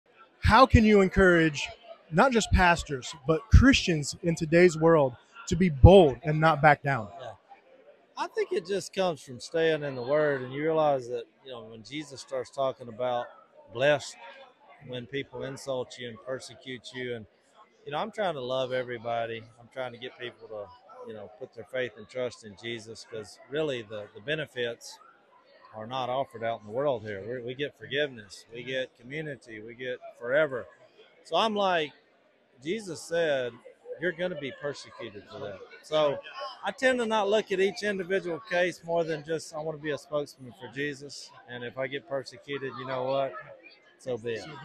Jase and Missy Robertson on the K Love Awards Red Carpet
Jase and Missy Robertson discuss how to be bold in your faith during the K-Love Fan Awards 2023 Red Carpet.